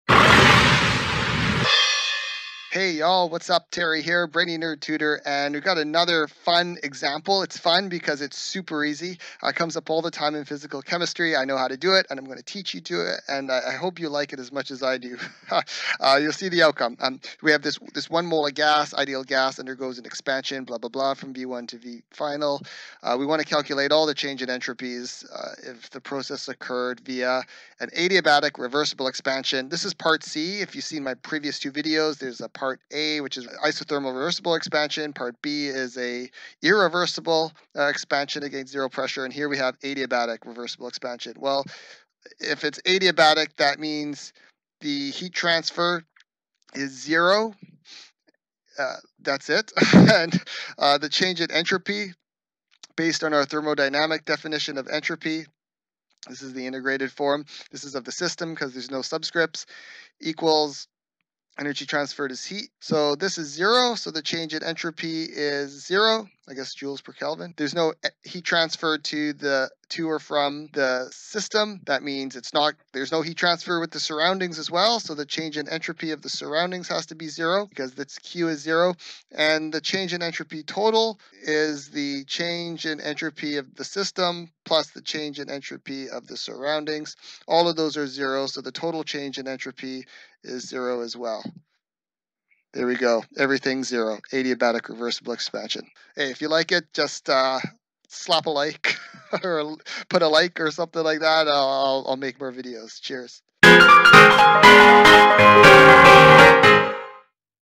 Entropy Change - 3/3 - Adiabatic Reversible Expansion of an Ideal Gas
Calculating the change in entropy comes up all the time in physical chemistry.

1) Isothermal Reversible Expansion of an Ideal Gas  (current video)